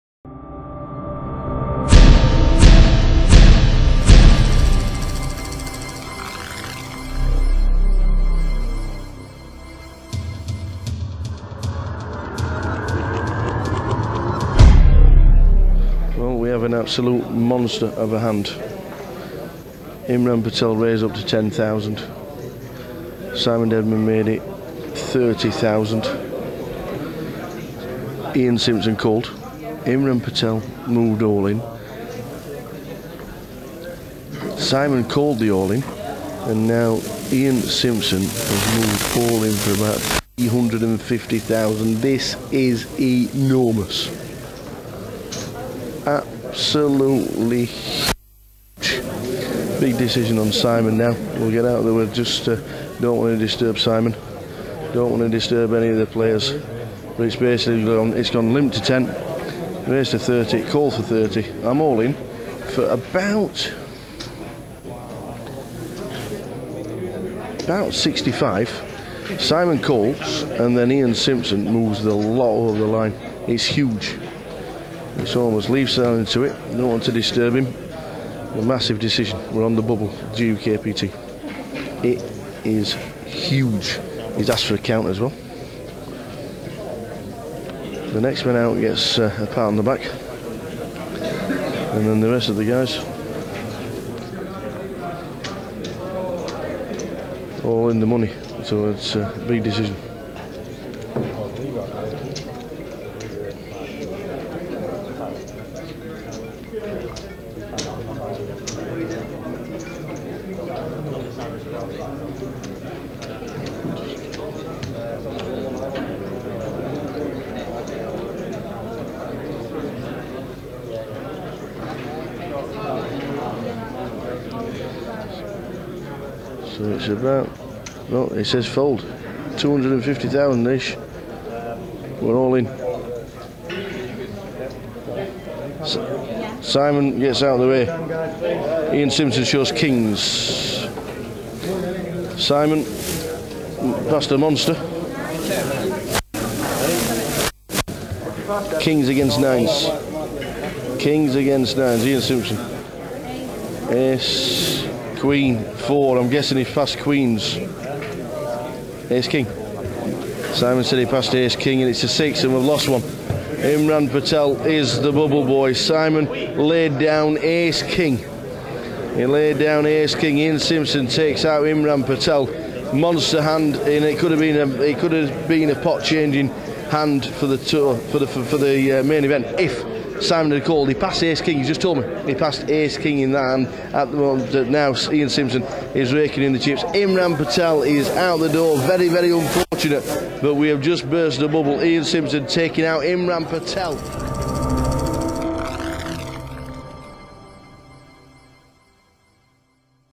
GukPT Manchester Day 2 - The Bubble Bursts
We see the money bubble burst as we lose Imran Patel